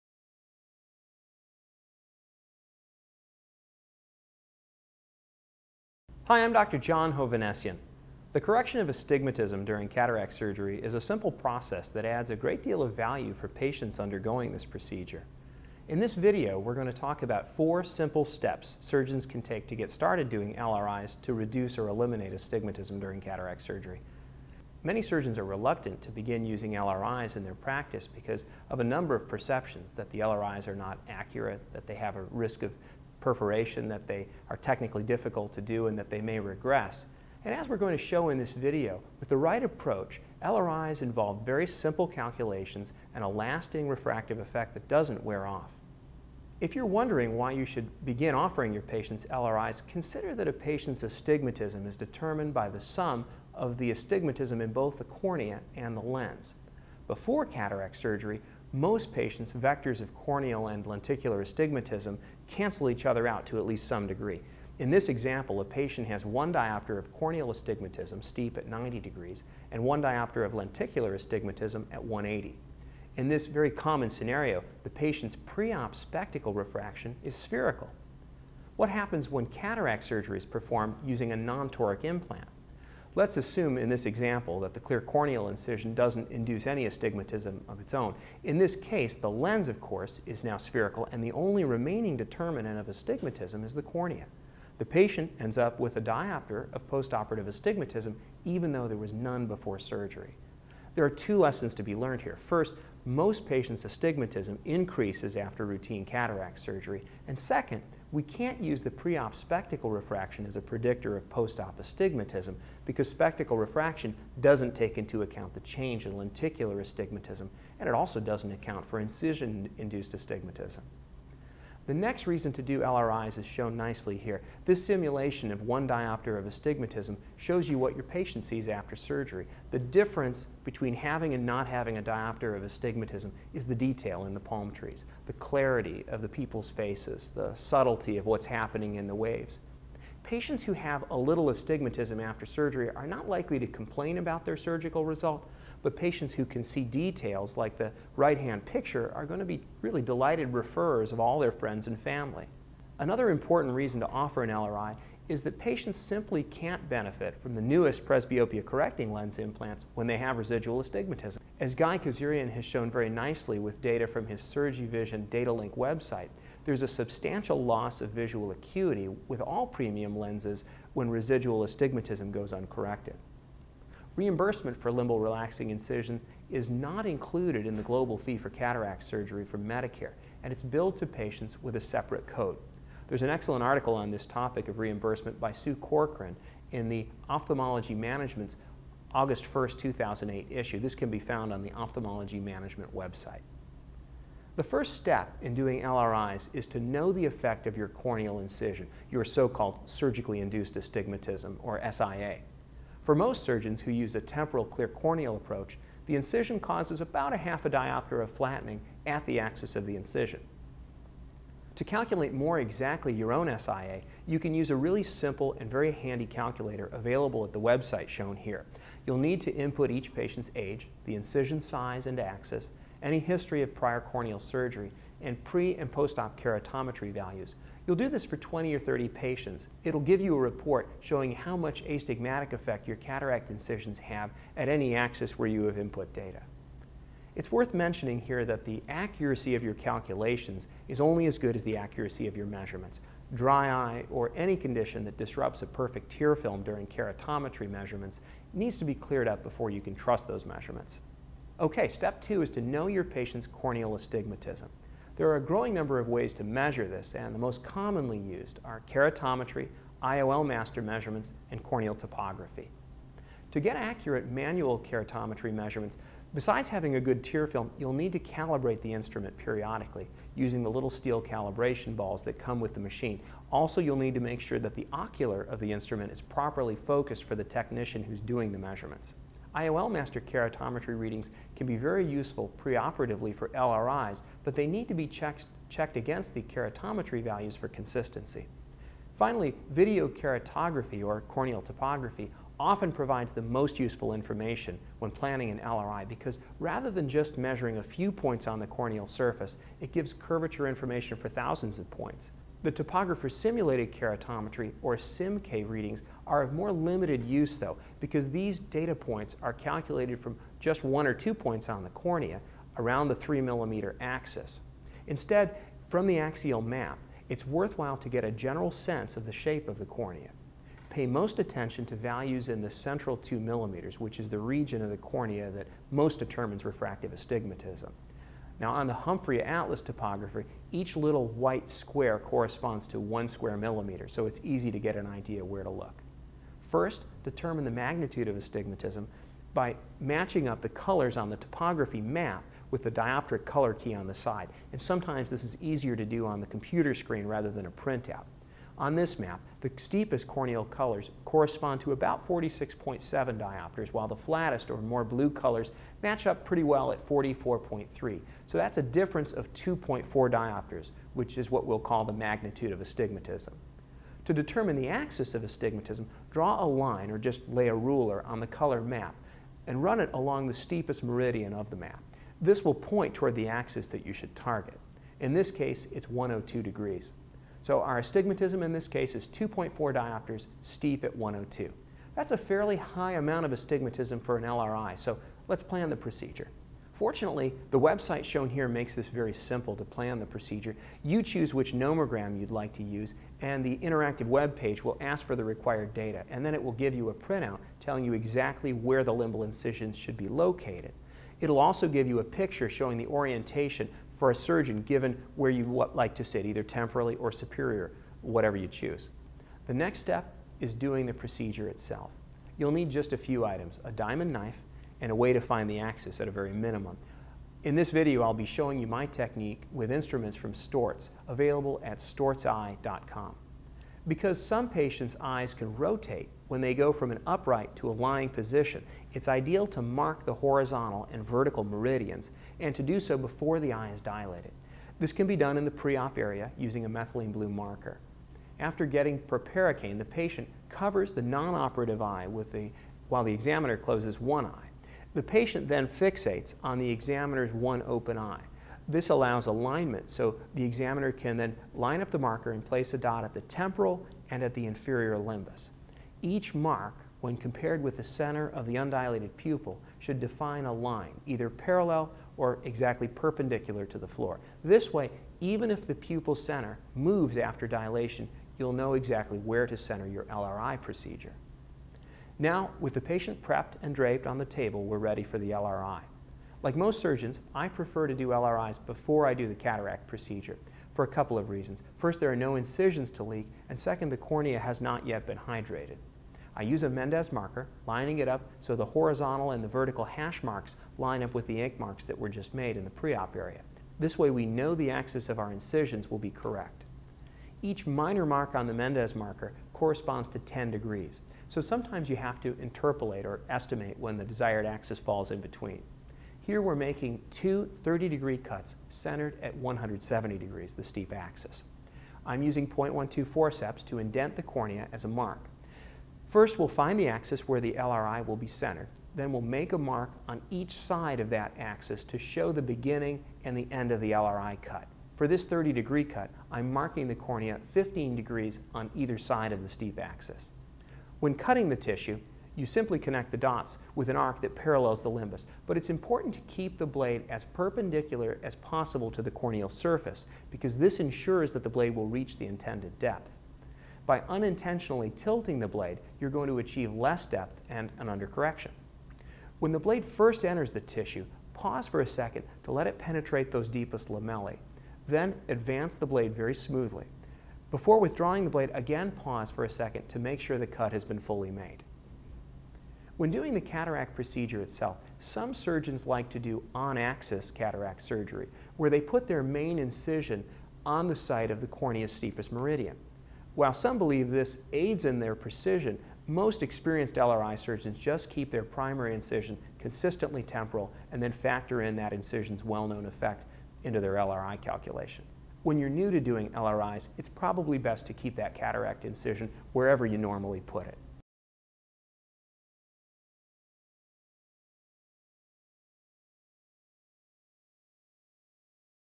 Astigmatism Correction - Cataract Surgery - Part 1
4 Steps to Astigmatism Correction During Cataract Surgery - Part 1: If you're a surgeon who is reluctant to start using premium lens implants, why not try doing limbal relaxing incisions (LRI's) as a first step in getting into refractive cataract surgery?